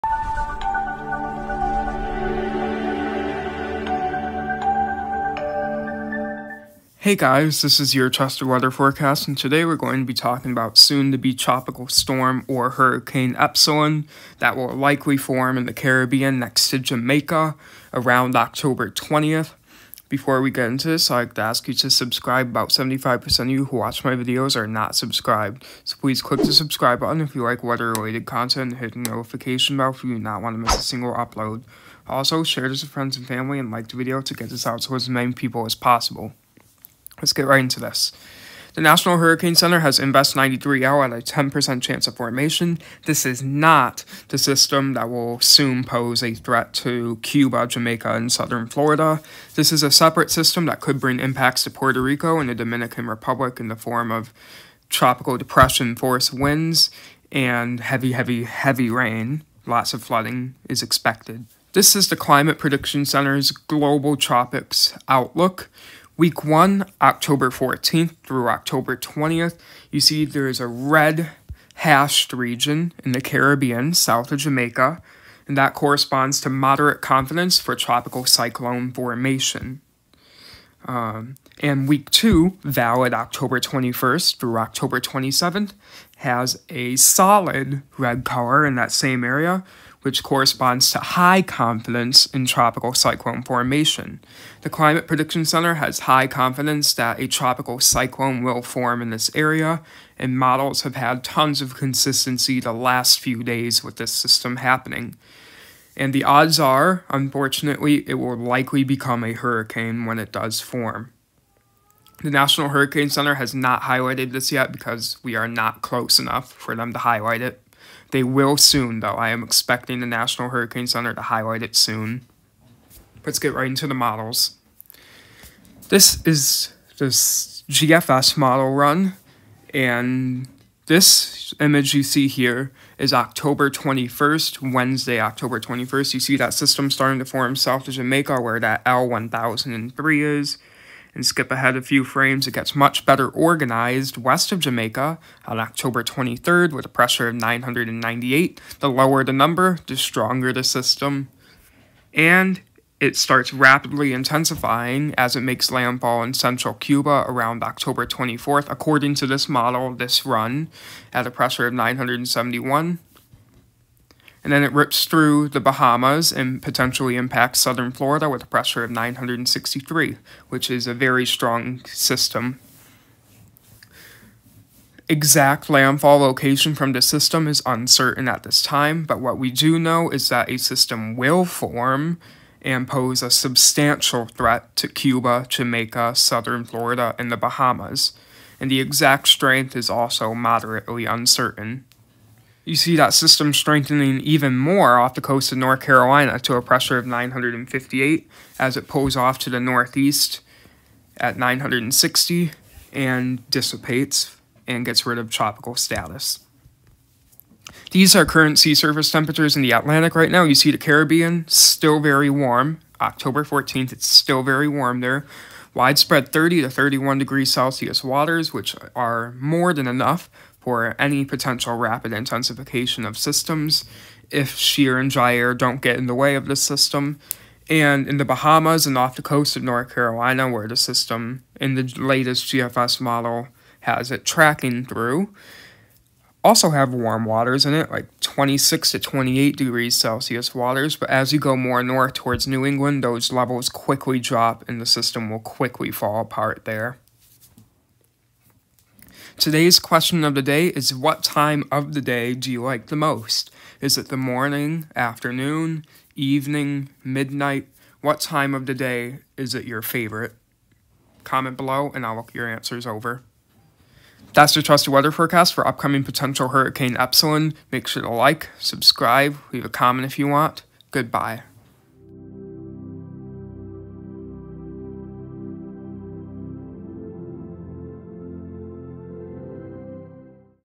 Potential Hurricane Epsilon
A tropical cyclone will likely form south of Jamaica within the next 10 days and potentially intensify into a hurricane before likely striking Cuba. This system which will likely be named Epsilon could make a second landfall on either southern Florida, or the Bahamas as a strong cyclone. Significant impacts are possible in Jamaica, Cuba, South Florida and the Bahamas as a result of this system if it forms. Impacts may be felt in North Carolina as well if it passes close enough.

Question of the day: What time of day do you like most?